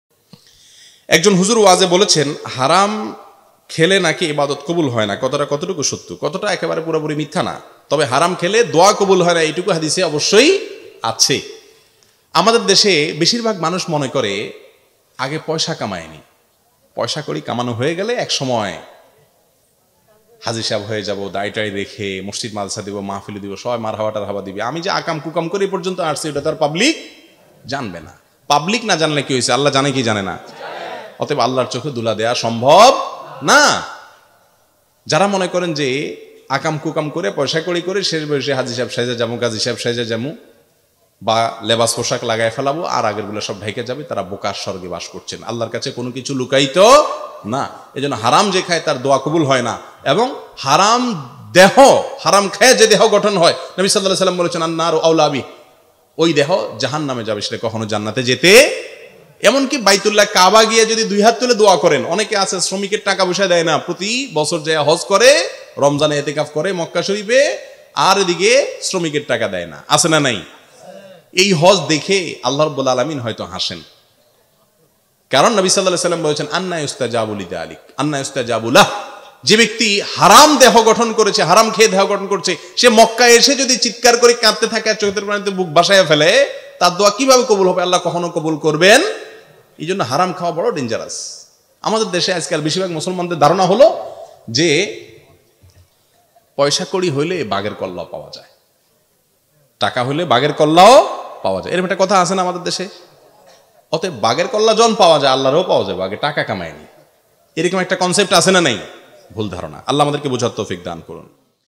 হারাম খেলে ইবাদত কবুল হবে কিনা । শায়খ আহমাদুল্লাহ প্রশ্ন উত্তর । Sheikh Ahmadullah
জীবন ঘনিষ্ট প্রশ্নের উত্তর - শায়খ_আহমাদুল্লাহ_প্রশ্ন_উত্তর. Sheikh Ahmadullah New Waz 2021. ইসলামিক প্রশ্ন উত্তর শায়খ আহমাদুল্লাহ. প্রশ্ন উত্তর পর্ব শায়খ আহমাদুল্লাহ.